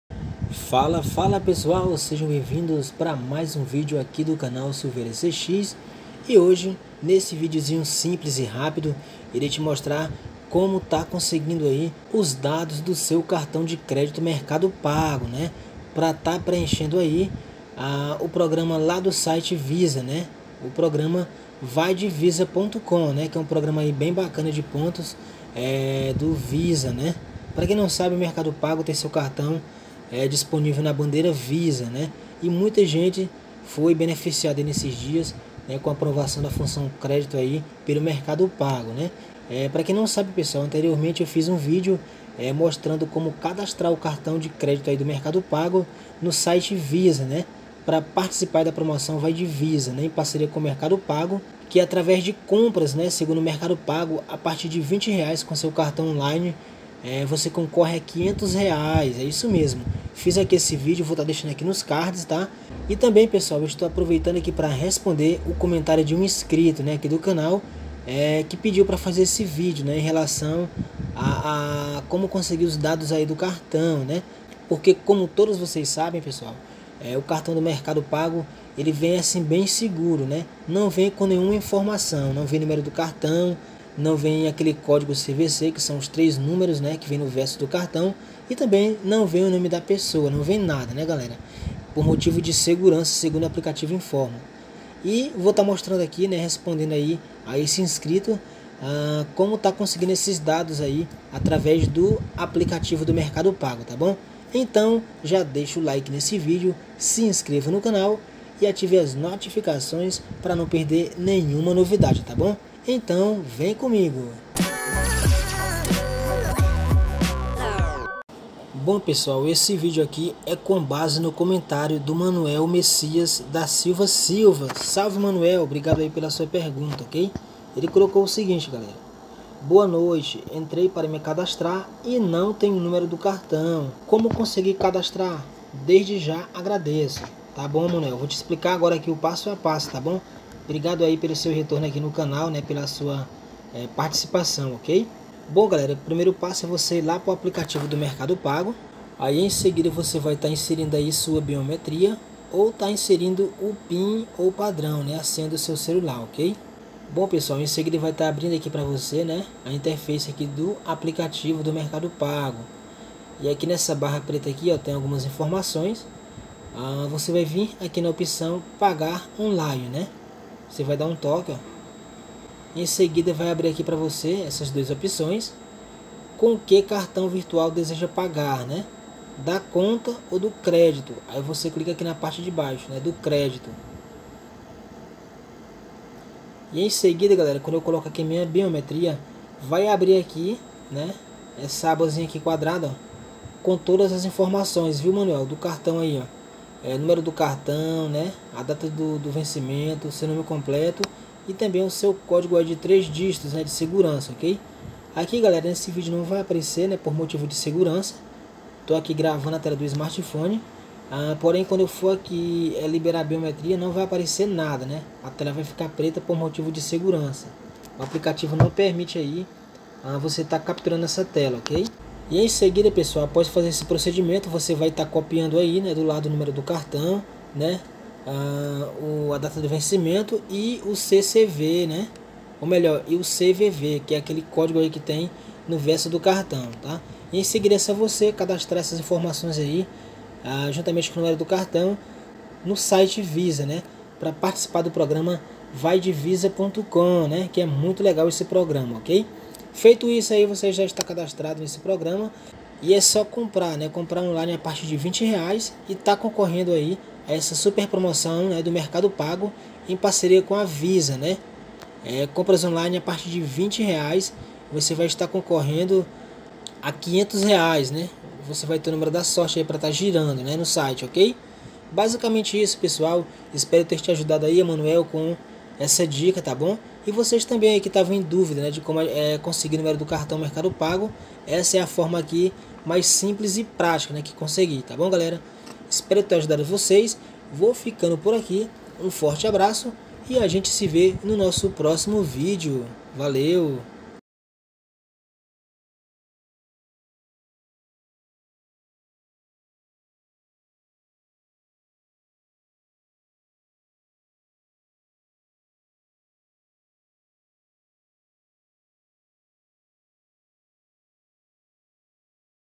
CARTÃO DE CRÉDITO MERCADO PAGO COMO CONSEGUIR O NÚMERO DO CARTÃO PARA CADASTRA NO SITE VISA
FALA PESSOAL BLZ! NESSE VIDEO RESPONDENDO A UM INSCRITO, VOU MOSTRA COMO CONSEGUIR AS INFORMAÇÕES DO CARTÃO DE CRÉDITO VIRTUAL DO MERCADO PAGO, ENTÃO VEM COMIGO...